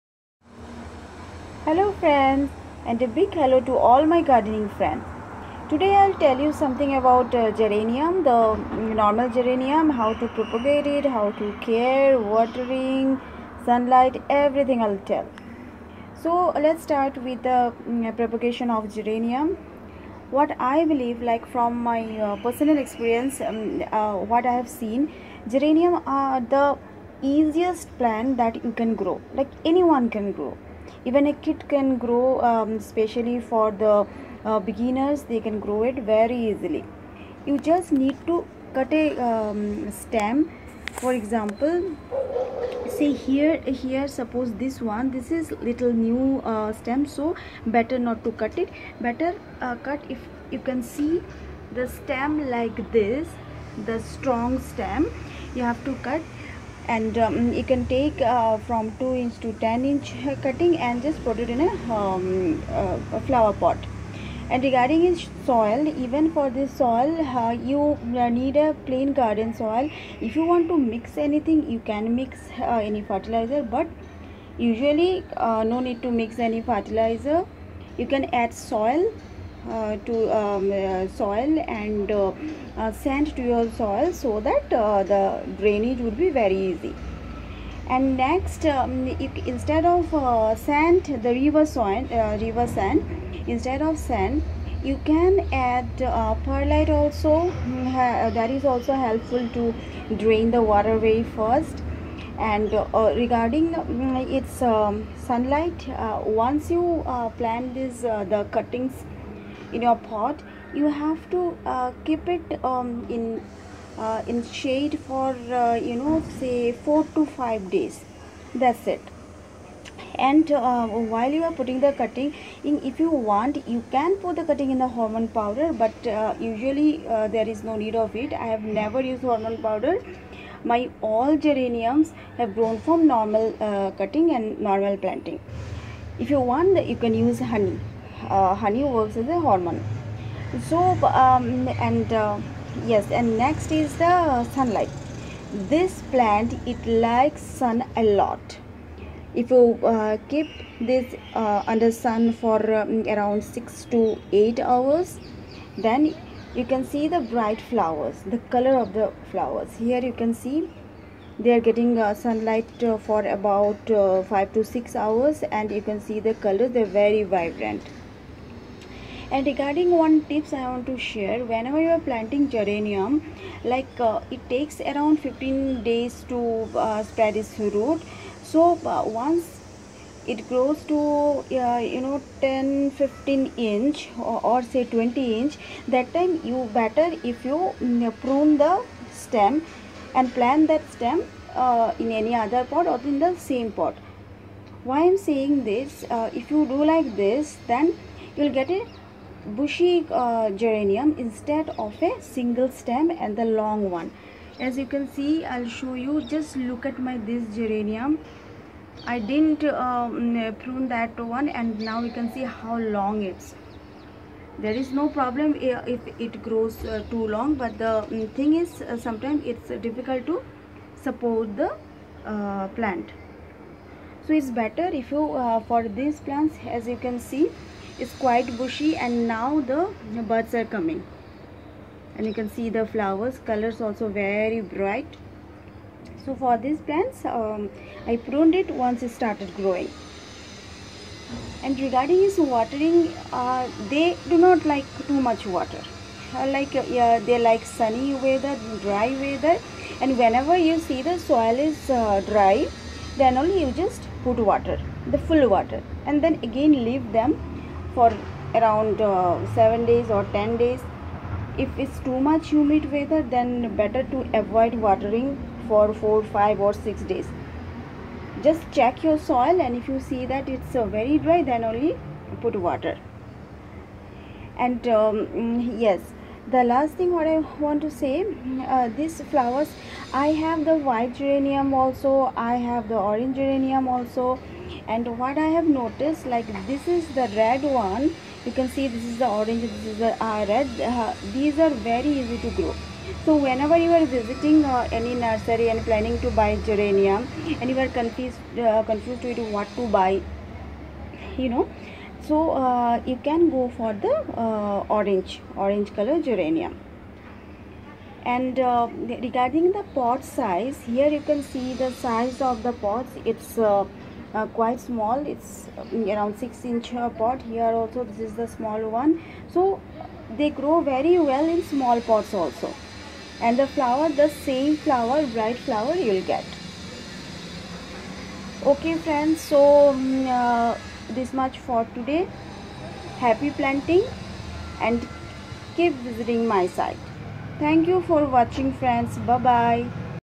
How to propagate and care Geranium II Geranium plant care II Anti -stress plant.
Geranium are the most easy plants to grow and care and rarely they get attacked by insects. Only thing we have to remember is we have to prune it when it starts growing so that we can make it bushy , otherwise it will become too long and sometime it is difficult to support it. When you prune it , plant that part in the same pot. Do not forget to pinch off the dead flowers to make it more healthy.